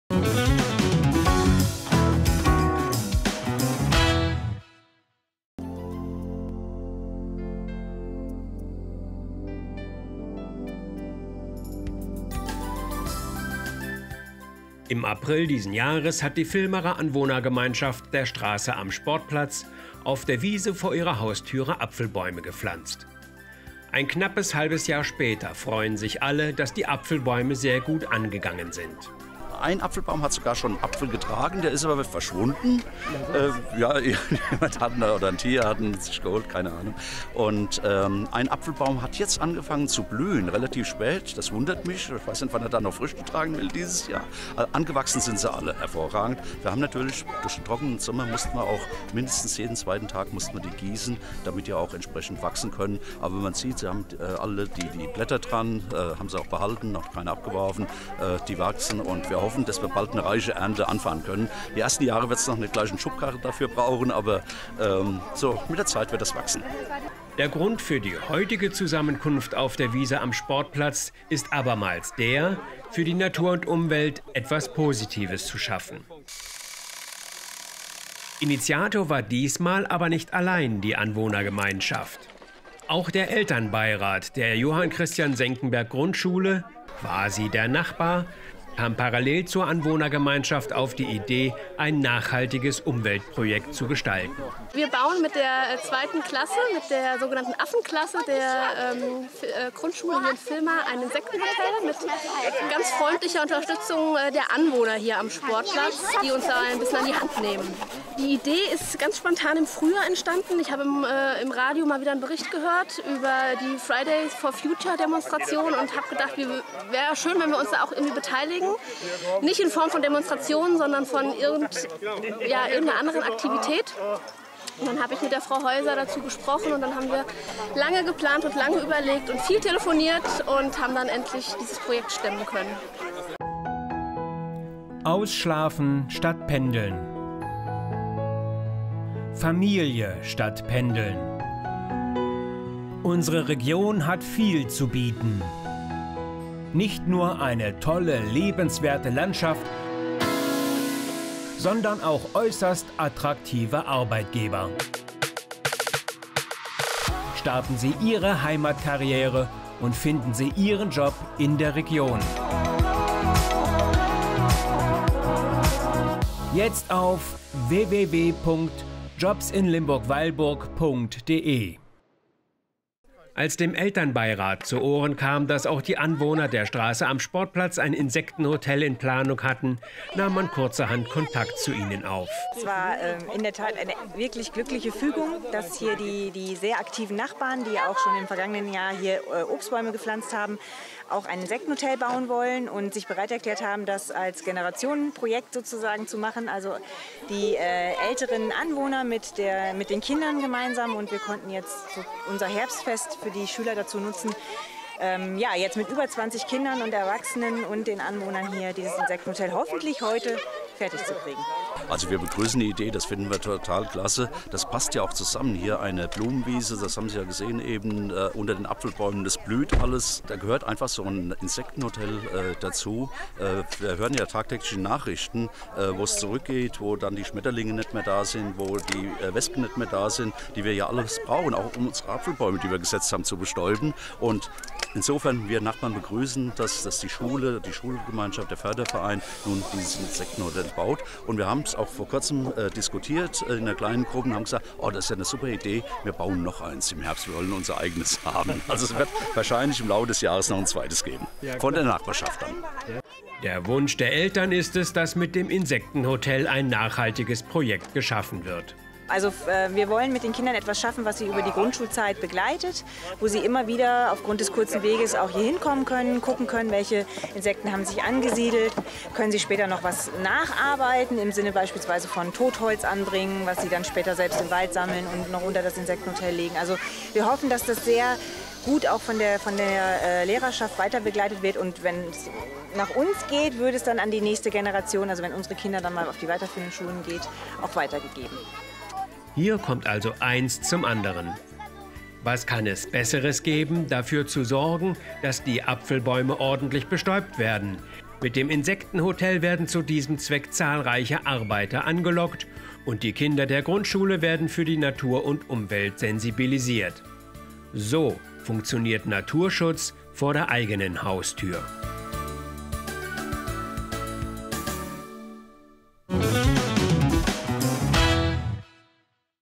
Insektenhotel der JC-Senckenberg Grundschule in Villmar
Die Schüler und der Elternbeirat der Klasse 2a der Johann-Christian-Senckenberg Grundschule in Villmar haben sich auf Ihre Art an der "Fridays for Future-Aktion" beteiligt. Auf der Wiese an der Straße "Am Sportplatz" in Villmar wurde ein Insektenhotel gebaut, das von den Grundschülern über ihre Schulzeit hinweg betreut und an die nachfolgenden Klassen weitergereicht werden soll. So wird das Überleben der Insekten sicher gestellt und der Biologie Unterricht zukünftig lebendiger gestaltet.